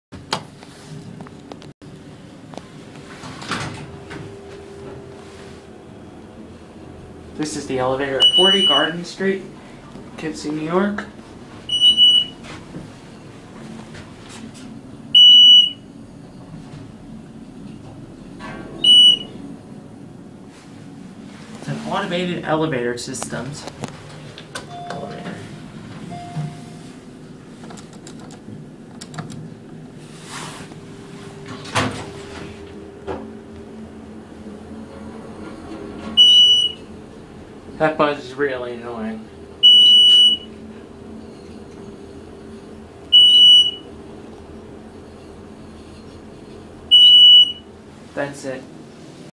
Automated Elevator Systems Traction Elevator At 40 Garden Street In Poughkeepsie NY
This is the Automated Elevator Systems traction elevator at 40 Garden Street in Poughkeepsie NY.